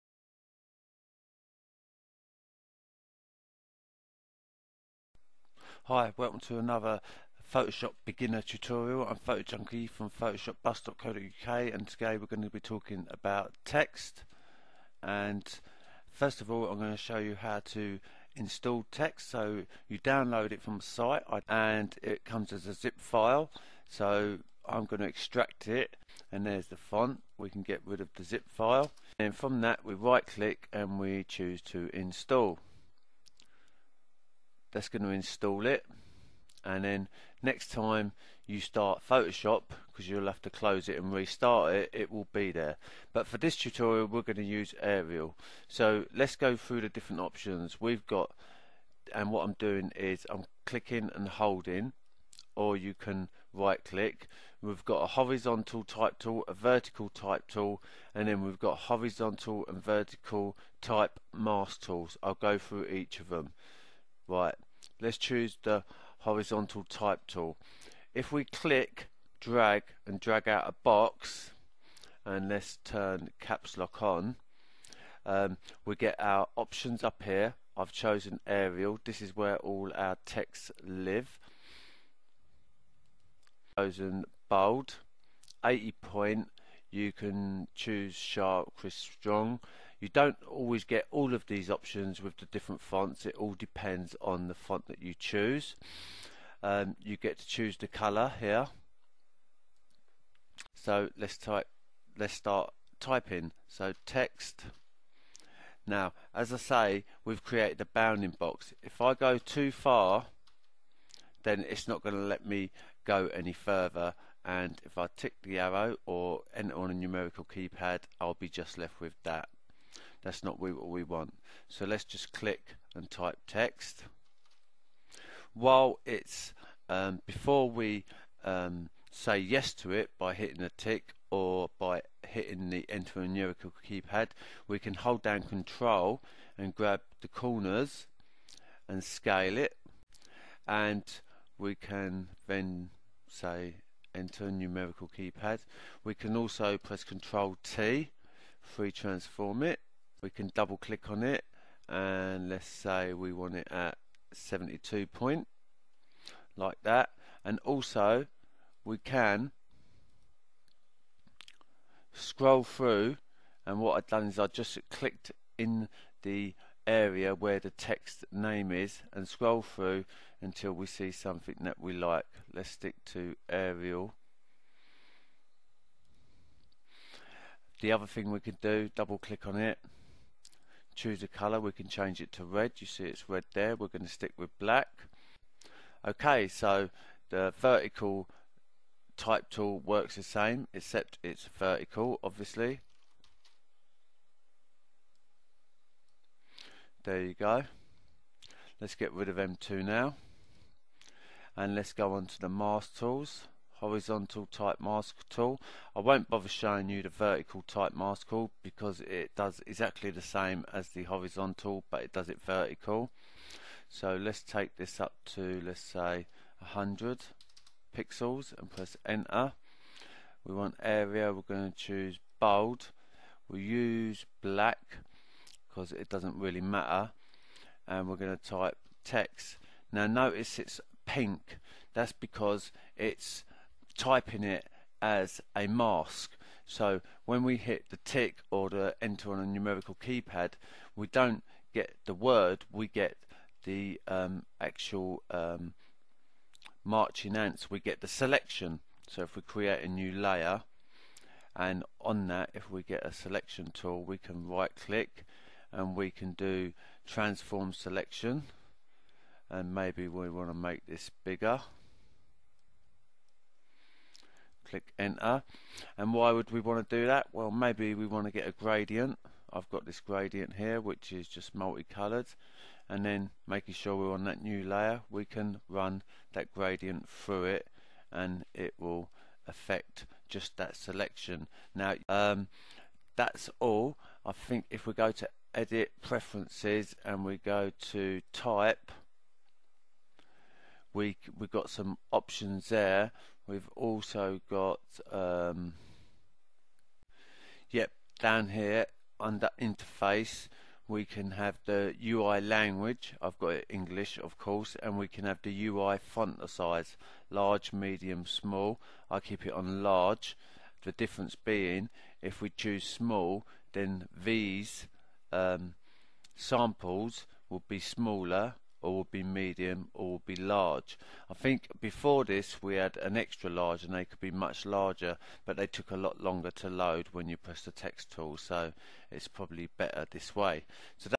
Photoshop Beginner Tutorial: Basic Text Options
Here is some basic techniques for using type/text in photoshop. I will show you how to install a font. Then I will show you the different type options, like horizontal/vertical and mask type tool. How to transform text, choose the different options from the options bar and how to change font preview size via preferences. So just real basic stuff for this beginner text. No character or paragraph options covered in this tutorial as that is a bit more advanced for a beginner tutorial. Hope you enjoy and learn. And comment if you like please.

For work file/s goto link above.